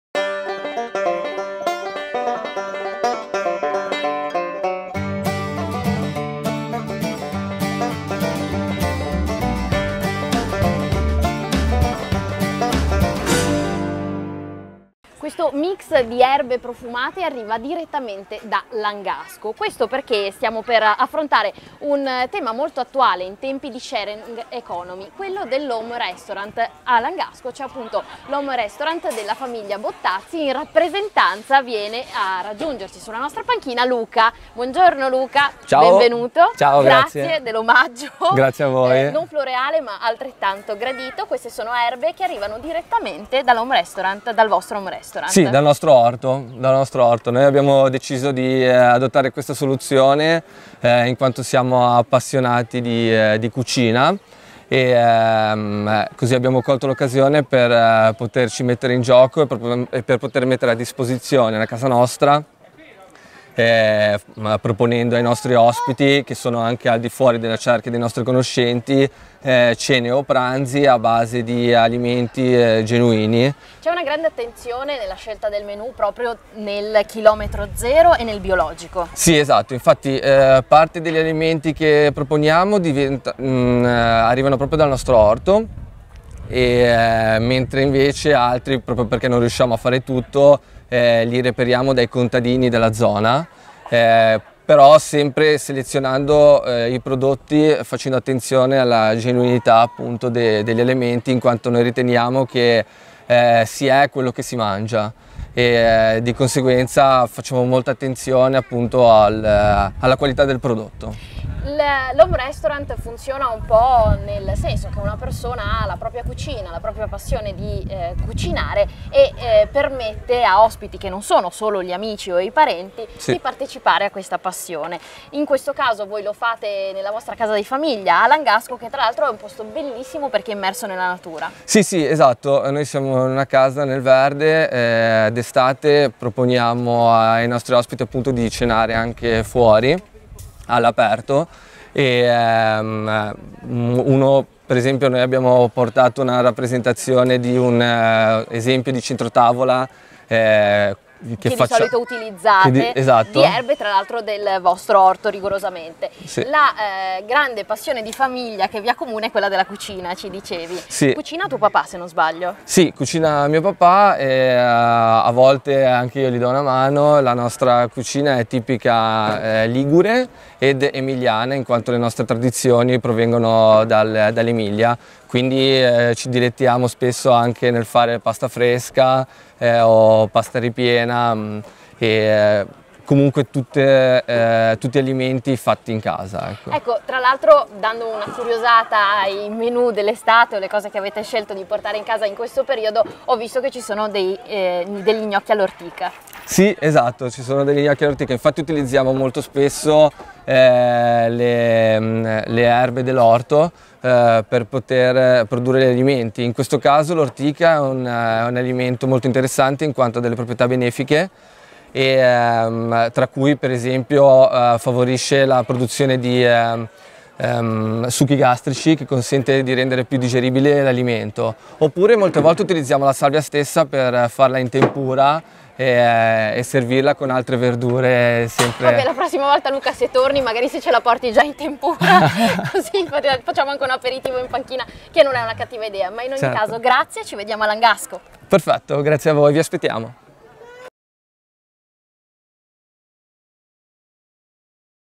Indovina chi viene a cena (all'home restaurant)
La passione di una famiglia (la cucina e i prodotti a km 0) che in tempi di sharing economy si trasforma in un home restaurant. Succede a Langasco, in casa Bottazzi.